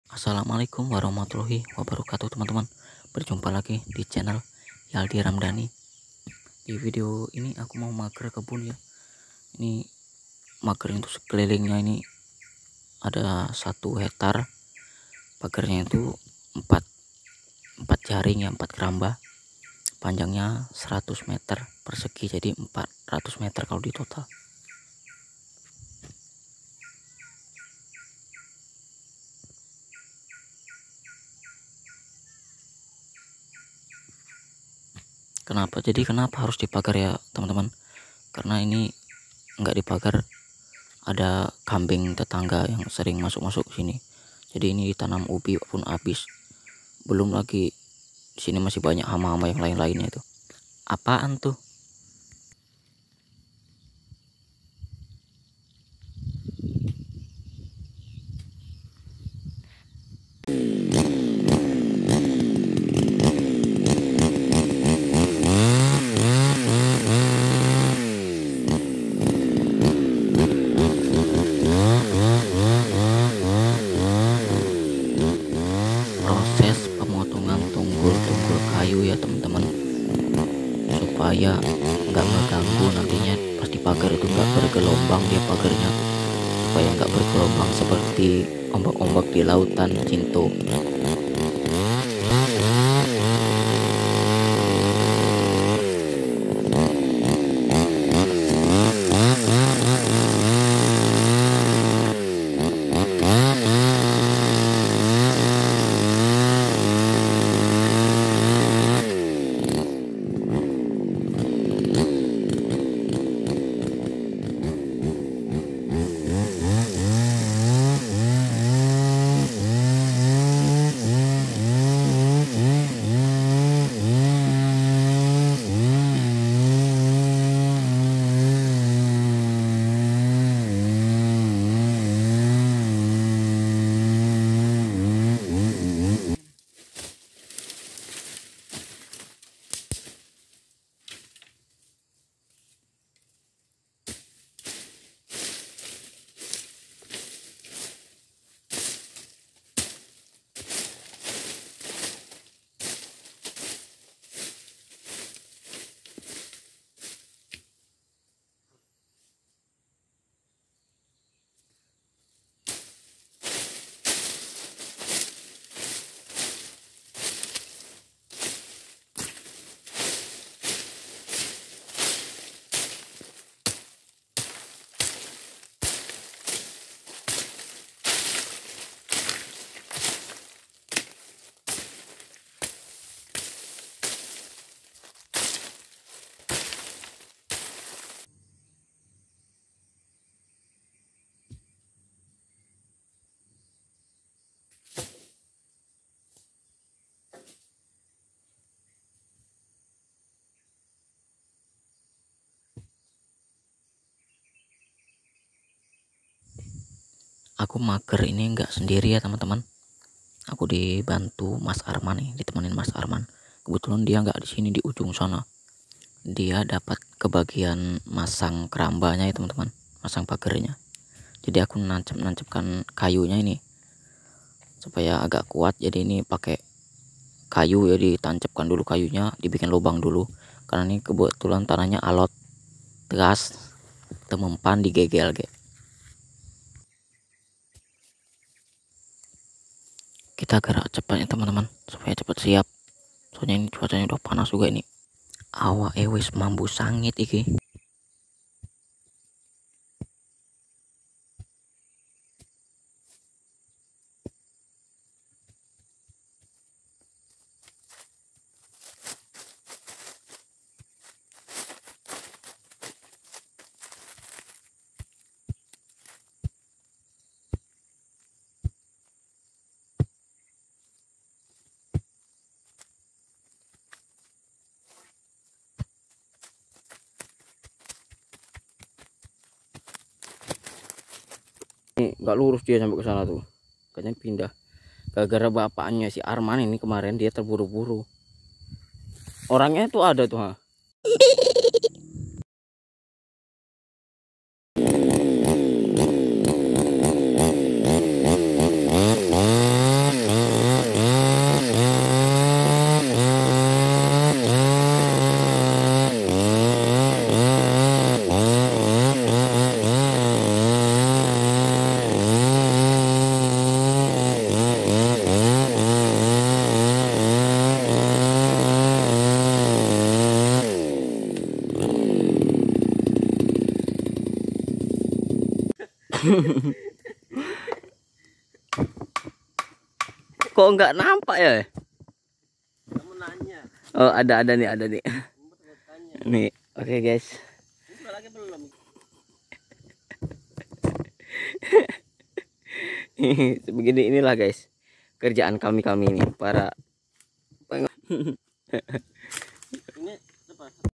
Magar kebun, mengunakan jaring keramba.!!
Proses memagar kebun luas 1h mengunakan
Jaring keramba lokasi riau,